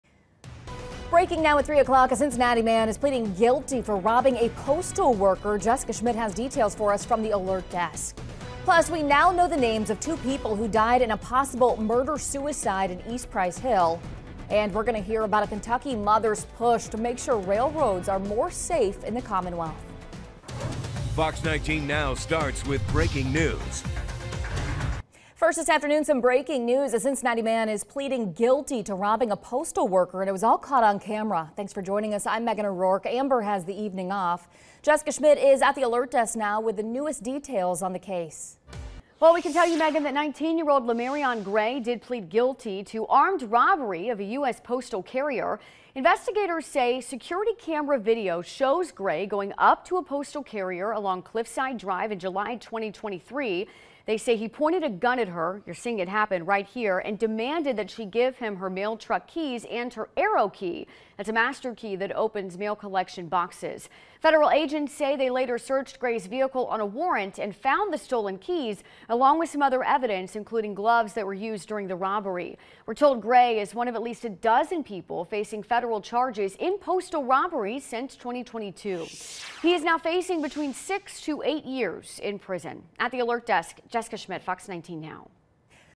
Man pleads guilty to mail carrier robbery that was caught on camera
FOX19 NOW at 3 p.m.